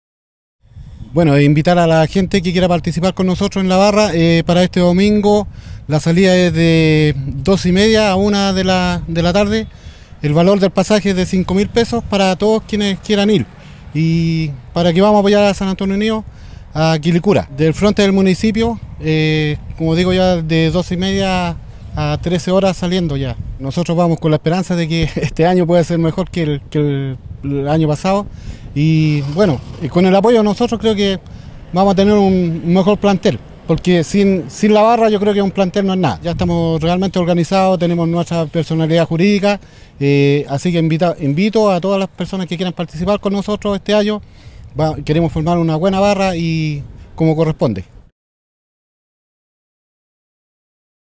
30 MAR 2012 BARRA_SAU.mp4
Este domingo a las 16 horas San Antonio Unido disputa su primer partido en el campeonato de tercera división, el cual se juega ante Deportes Quilicura en la región metropolitana, y la barra del SAU prepara su viaje a la capital, por esto Raúl Acevedo invita a los hinchas a apoyar al elenco del puerto, donde un bus partirá a las 12:30 en el frontis de la municipalidad local.